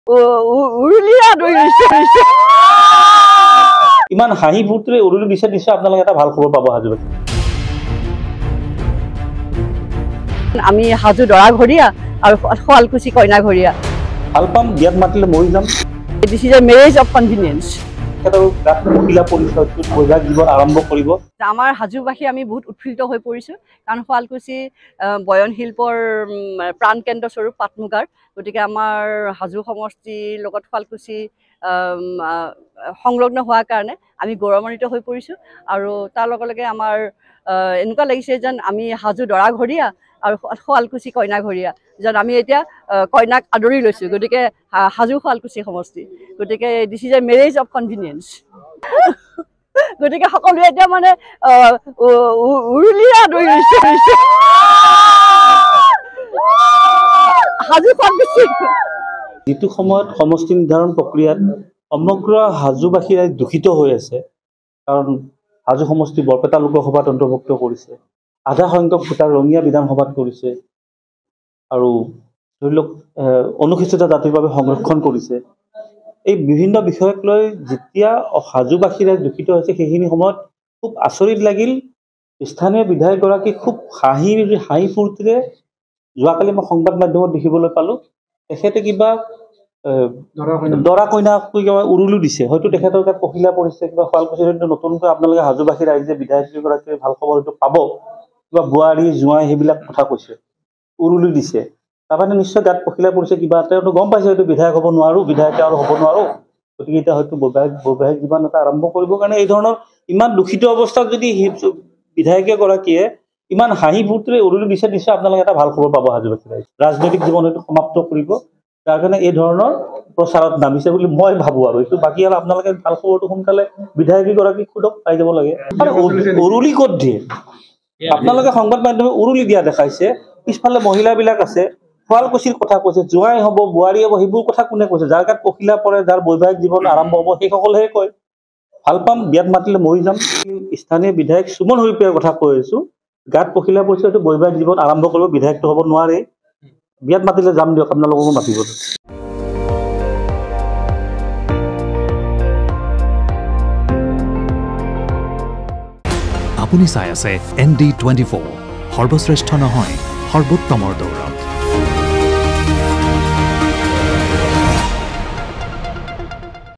Suman Haripriya News: সুমন হৰিপ্ৰিয়াৰ গাত পখিলা পৰিছে, শীঘ্ৰে বিয়াত বহিব : দুলু আহমেদ
'সুমন হৰিপ্ৰিয়াৰ গাত পখিলা পৰিছে, শীঘ্ৰে বিয়াত বহিব।' নিমন্ত্ৰণ পালে বিয়া খাবলৈ যাব দুলু আহমেদে। সমষ্টি পুনঃনিৰ্ধাৰণক লৈ কৰা সুমন হৰিপ্ৰিয়াৰ মন্তব্য সন্দৰ্ভত কিয় এইদৰে ক'লে AJP নেতা দুলু আহমেদে?